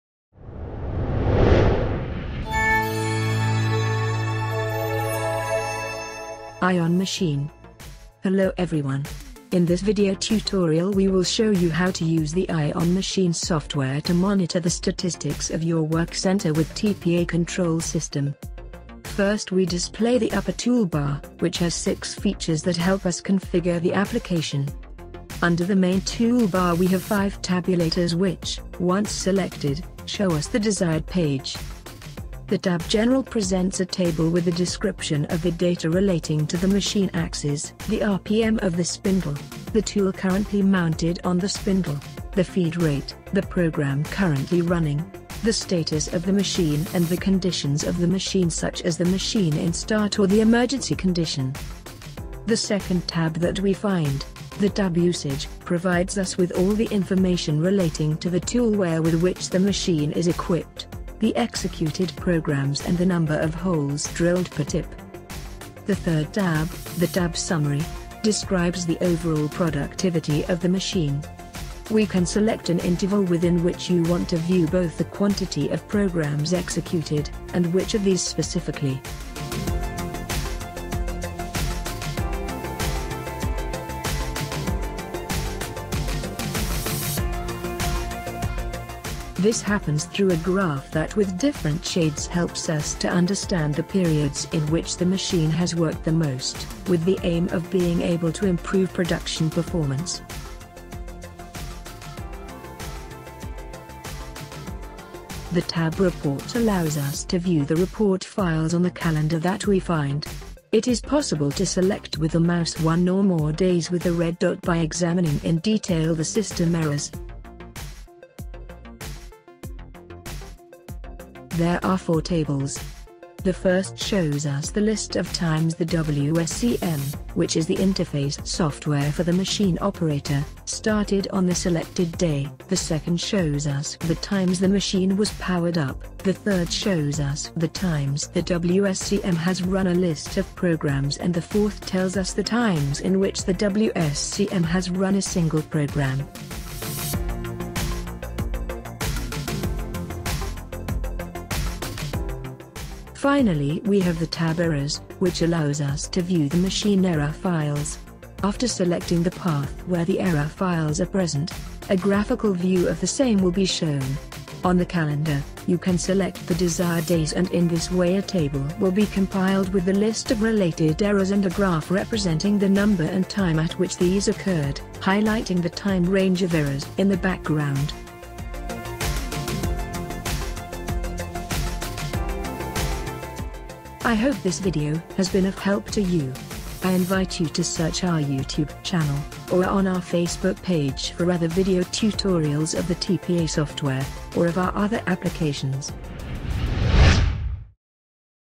Tpa Eye On machine
In this video tutorial we will show you how to use the Eye On Machine software to monitor the statistics of your work center with Tpa control system. If you are interested we invite you to visit our